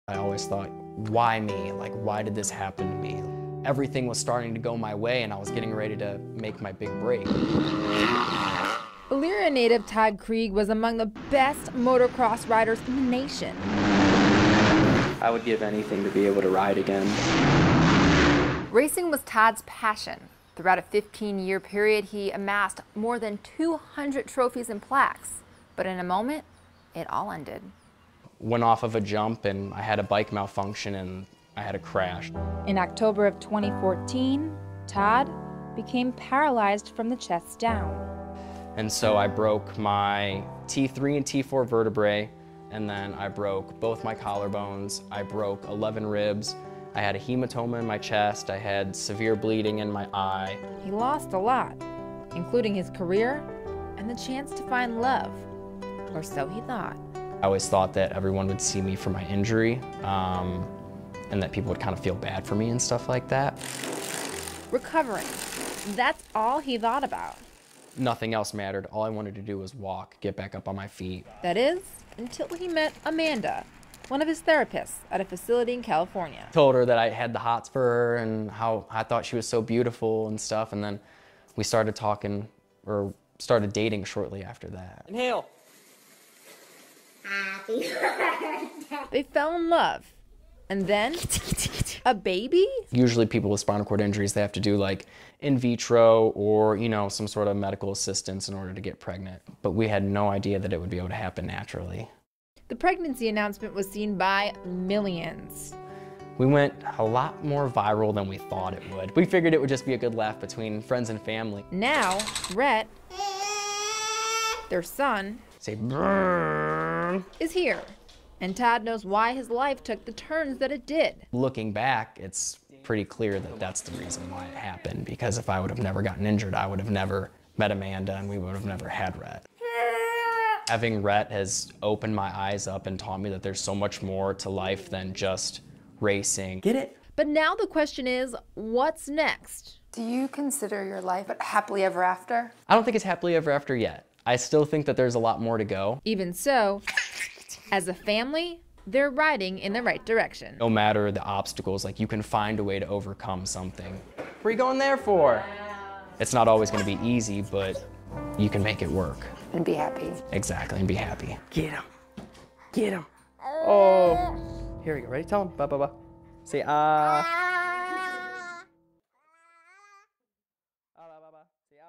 Motocross rider from Elyria finds love and happiness after paralysis
Snatching triumph from tragedy